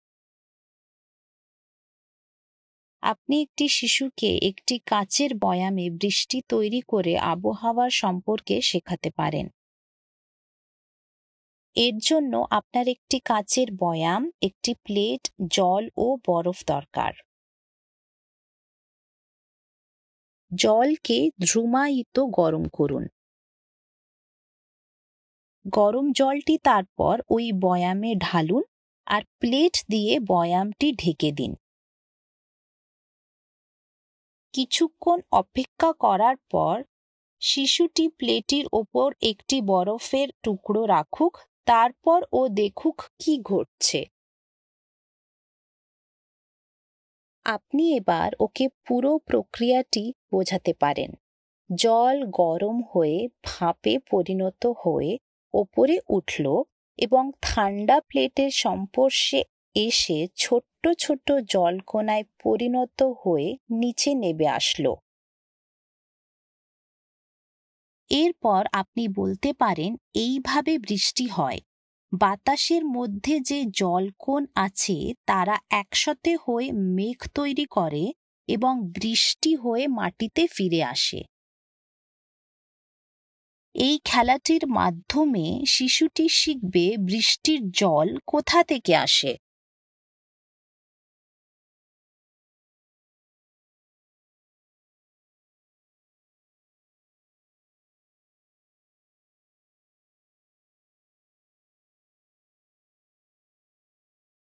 বৃষ্টি তৈরী - Making Rain (Bengali)
আপনি একটি শিশু কে একটি কাঁচের বয়ামে  বৃষ্টি তৈরী করে আবহাওয়া সম্পর্কে শেখাতে পারেন। তার জন্য আপনার  একটি কাঁচের বয়াম,একটি প্লেট,জল ও বরফ দরকার।

টিচ ফর লাইফ আমাদের বাচ্চাদের আরও ভাল শিক্ষিত করবার জন্য় এবং বিশ্বে আশা তৈরি করবার জন্য়একটি বিশ্বব্যাপী আন্দোলন।

Bengali translation by Poulomi Adhikary and narration by Seema Gangakhedkar.

You can help a child learn about weather by making rain in a jar. You will need a glass jar, a plate, water, and ice.